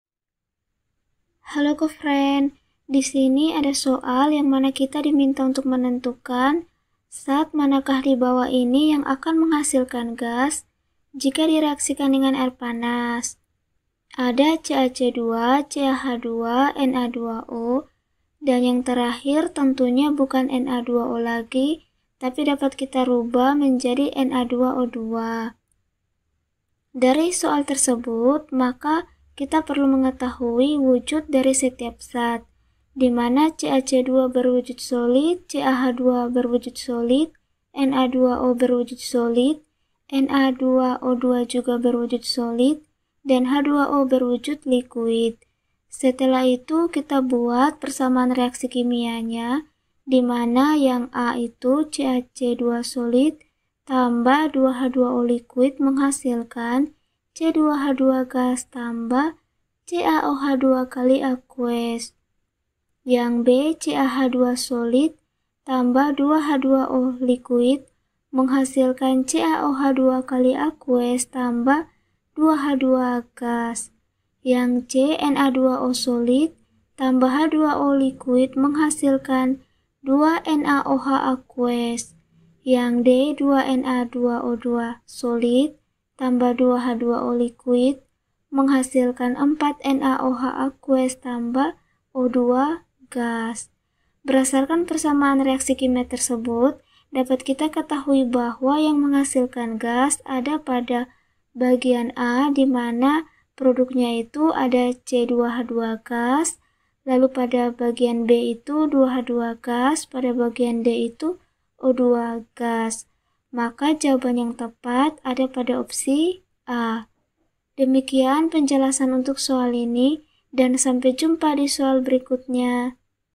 Zat manakah di bawah ini yang akan menghasilkan gas jika direaksikan dengan air panas a. CaCz C. ...
Zat manakah di bawah ini yang akan menghasilkan gas jika direaksikan dengan air panas a. CaCz C. NazO b. CaHz d. NazO Yang benar adalah a. a, b, € d. b, d e: a,d b_ e.d a, C

Topik atau Materi: Interaksi Antar Molekul - Ikatan Kimia, Bentuk Molekul, dan Interaksi Antarmolekul - Kimia Anorganik - Kimia Kelas 10 SMA - KTSP

Sumber Soal: Bank Soal Kimia SMA, KTSP, SND 2009 , N/A

00:00 : Bedah Soal
01:00 : Konsep, rumus dan pengertian Interaksi Antar Molekul
01:18 : Langkah penyelesaian soal
01:03 : Jawaban akhir
01:33 : Penutup